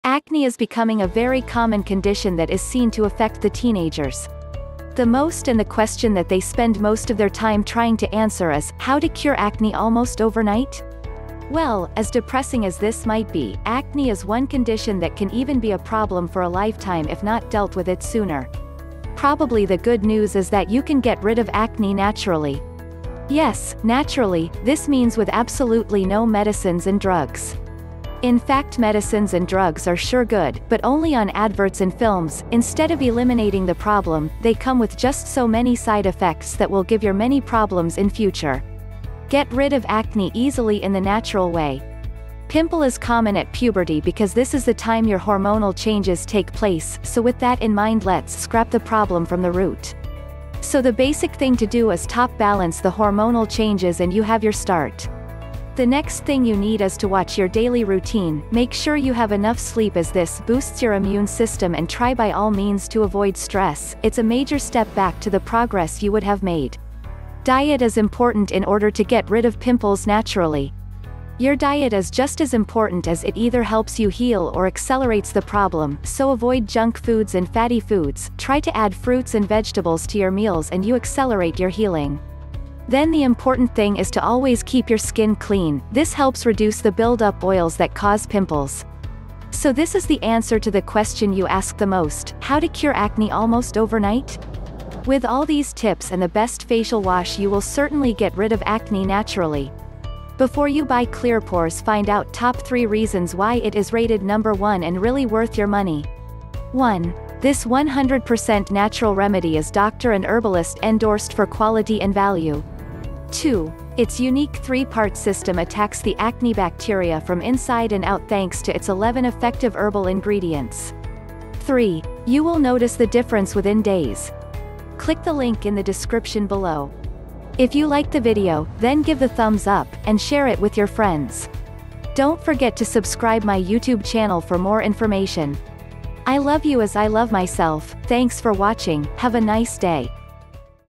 How to Cure Acne Almost Overnight - This is the Best Way to Get Rid of Acne Naturally!
Acne is becoming a very common condition that is seen to affect the teenagers. The most and the question that they spend most of their time trying to answer is, 'how to cure acne almost overnight?' Well, as depressing as this might be, acne is one condition that can even be a problem for a lifetime if not dealt with it sooner. Probably the good news is that you can get rid of acne naturally. Yes, naturally, this means with absolutely no medicines and drugs. In fact medicines and drugs are sure good, but only on adverts and films, instead of eliminating the problem; they come with just so many side effects that will give your many problems in future.

Get Rid Of Acne Easily In The Natural Way

Pimple is common at puberty because this is the time your hormonal changes take place, so with that in mind let's scrap the problem from the root. So the basic thing to do is top balance the hormonal changes and you have your start. The next thing you need is to watch your daily routine, make sure you have enough sleep as this boosts your immune system and try by all means to avoid stress; it's a major step back to the progress you would have made.

Diet is important in order to get rid of pimples naturally

Your diet is just as important as it either helps you heal or accelerates the problem, so avoid junk foods and fatty foods, try to add fruits and vegetables to your meals and you accelerate your healing. Then the important thing is to always keep your skin clean, this helps reduce the build up oils that cause pimples. So this is the answer to the question you ask the most, How to cure acne almost overnight?

With all these tips and the best facial wash you will certainly get rid of acne naturally.